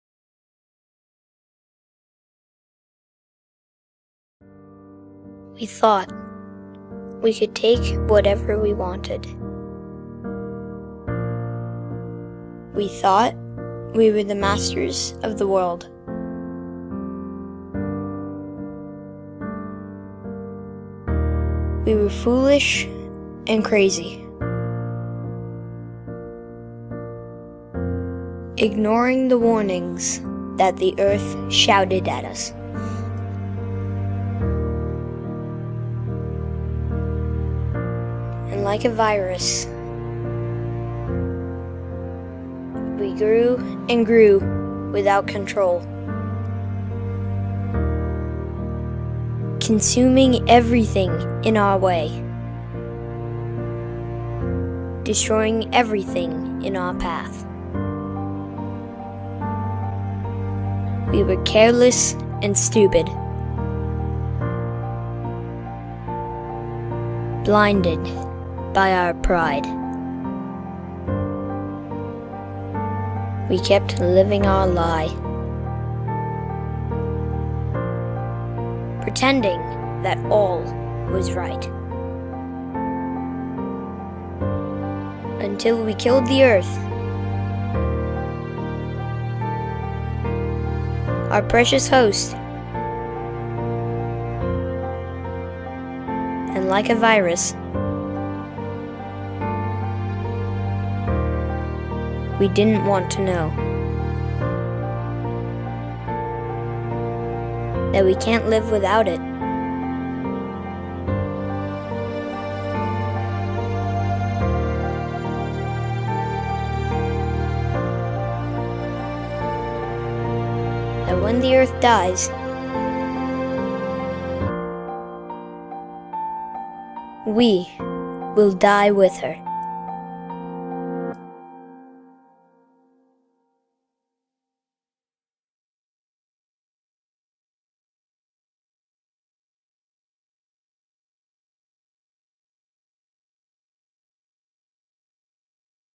Apocalypse Revisited
This is the second version of a short animation I created in a week
for the Post-Apocalypse Kitbash3D contest - March 2020

Here I replaced the narrator's synthetic voice by the lovely voice of Lucas Moras, Cris Moras son, adding emotional content to the video.

Kitbash 3D provided the Post Apocalypse Mini Kit that contains all the buildings present in the video, and I used the vehicle models of my "Doomsday vehicles combo" and the characters from my "Survivors" Pack, both available in Reallusion's Content Store.

Additional props have been included from different sources.

I did all the sets and animations in iClone and I rendered the scenes using the experimental iClone 8 - Alpha version provided for Reallusion to test their upcoming new features like the Volumetric lights, extensively used in the video.

Narrator’s voice by Lucas Moras

As usual I edited the video in Vegas Pro using the filters from  FX-Home Hitfilm Ignite pack.